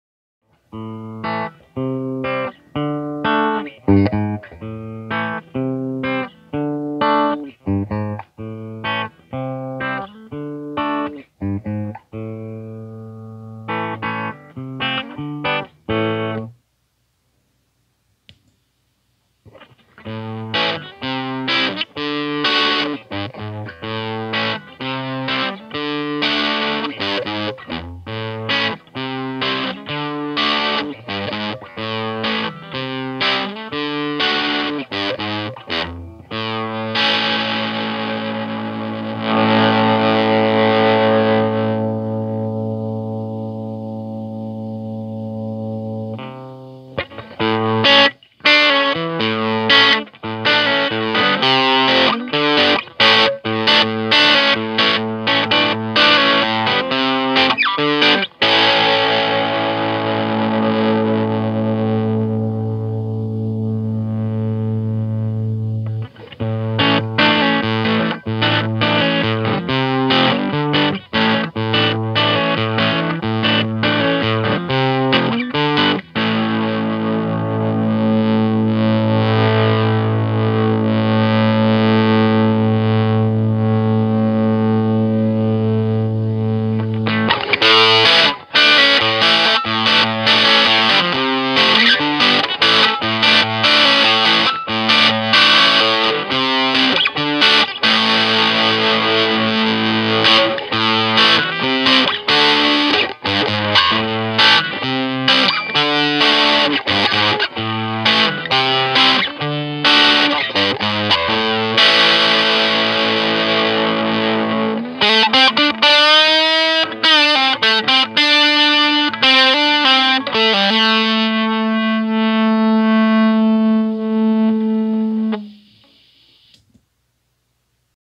Schu-Tone 1484 Overdrive
The Schu-Tone 1484 is an interpretation of the Silvertone 1485 amp preamp in pedal form.  Internally converted to 18V, this gives you the push to really bring your tones to life.  From a dirty boost all the way to a heavy grind, this pedal has your overdrive needs covered.  Add in a two band EQ that allows you to dial in the perfect amount of bass and treble, this is a super versatile overdrive.